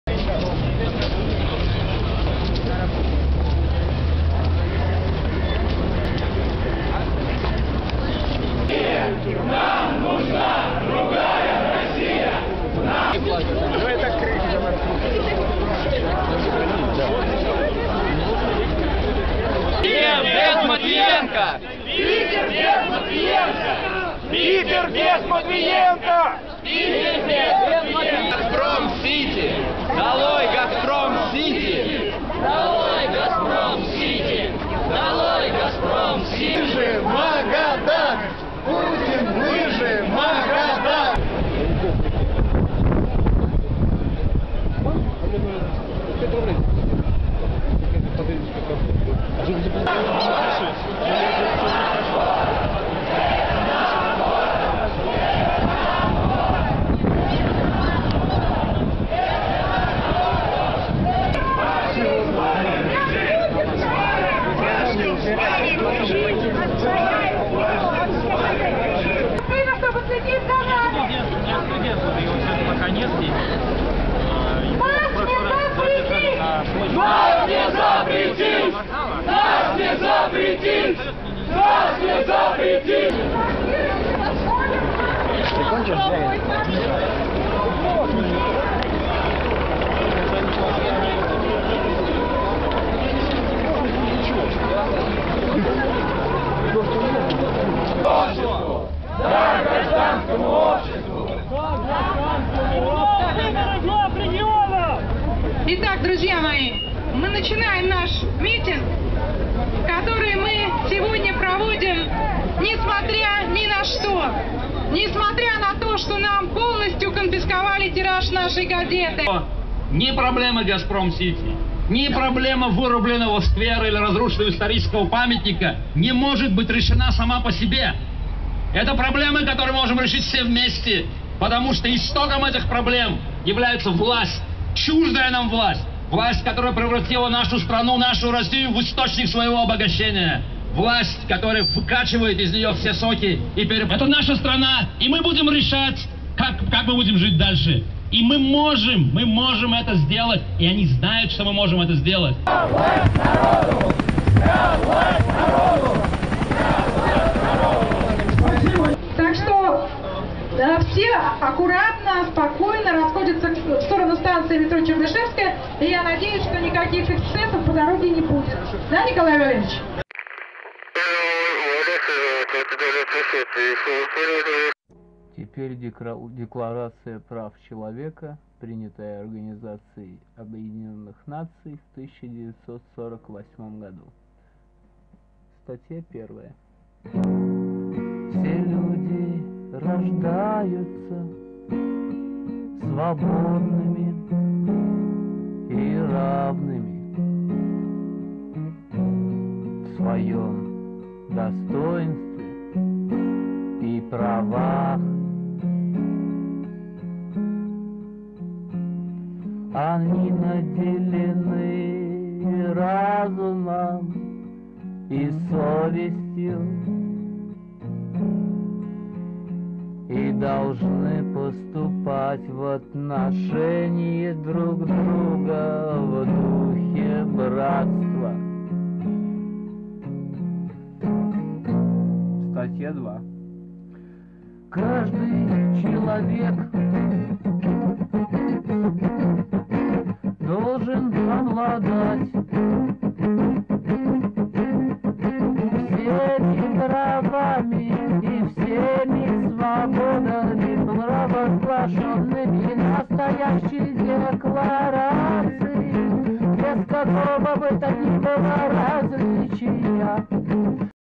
March of the Dissenters: 9 June 2007/ St. Petersburg
Despite having had the entire 160,000-copy run of their newspaper confiscated by the "security force," the March of the Dissenters hit the sunny streets of Petersburg to voice their disapproval of the regime. Music by legendary outsider artist Valery Cherkasov